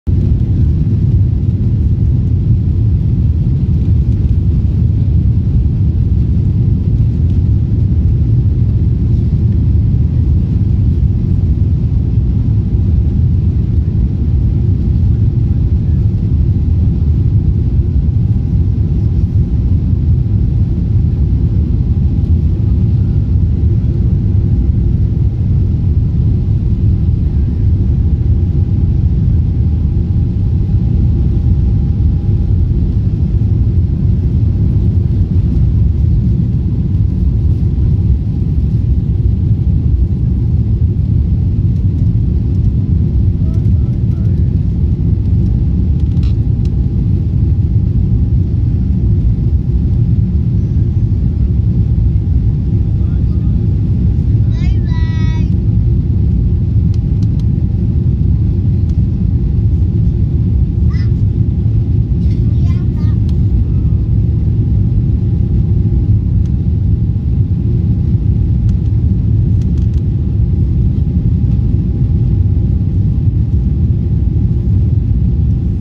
Despegando de Paris hacia Tokio Boeing 777-300 de Air France.
Despegando de Paris hacia Tokio en un Boeing 777-300 de Air France.

SÍGUEME